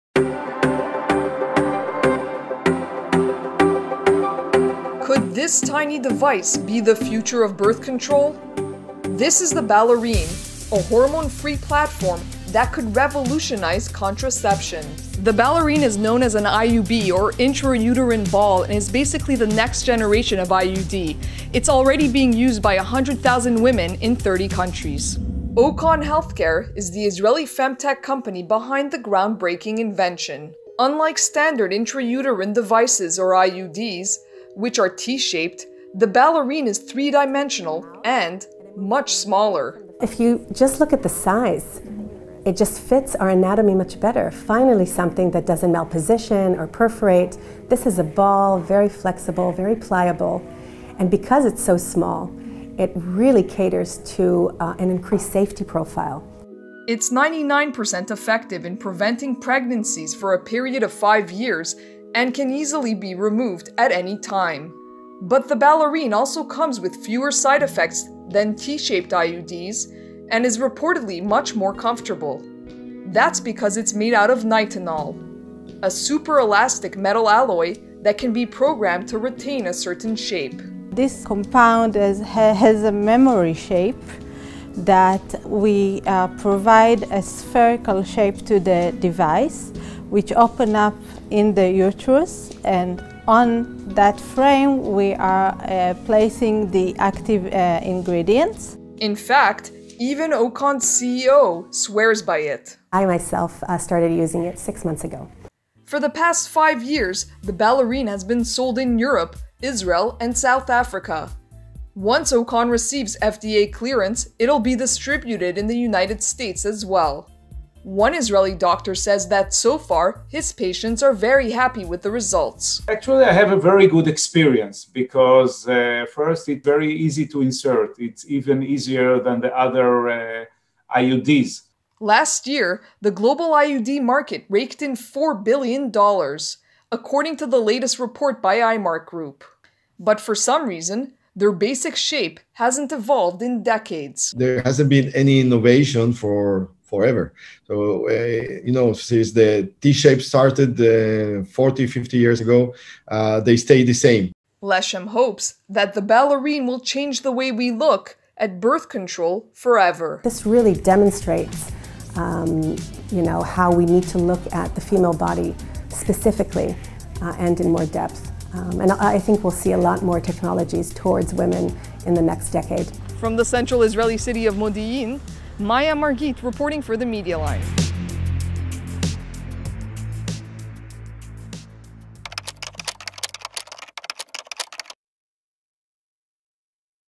Israeli Femtech Startup Invents Next Generation of Birth Control
Israeli femtech startup OCON Healthcare has developed a revolutionary intrauterine platform that prevents pregnancy and which can also be used to treat abnormal uterine bleeding.